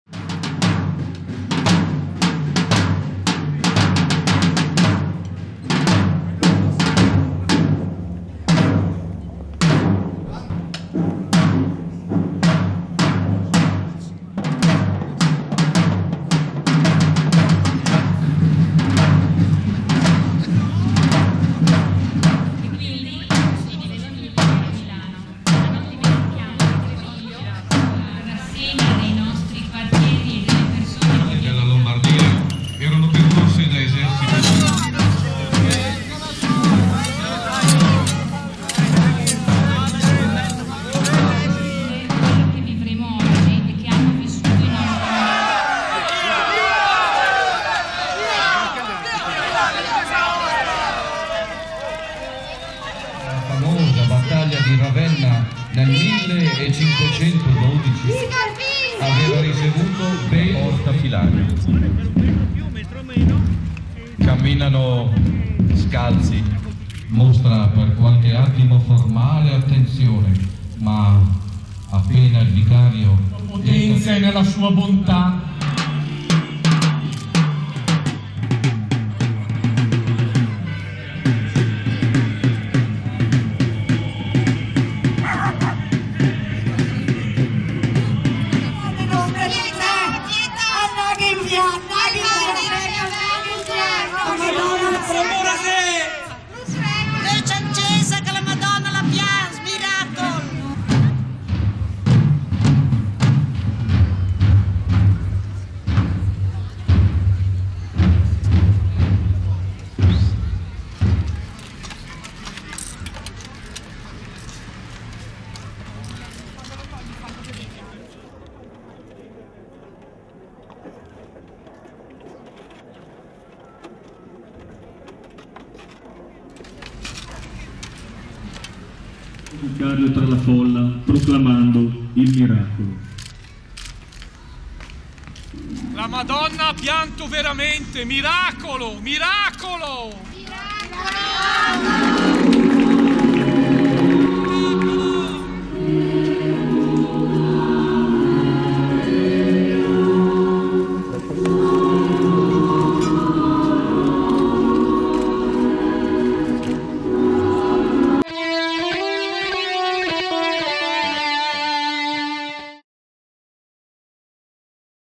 Miracol si grida!
Le immagini salienti della rievocazione storica del Miracolo della Madonna delle Lacrime , organizzata dalla Pro Loco di Treviglio ha visto migliaia di persone riversarsi nel centro della città.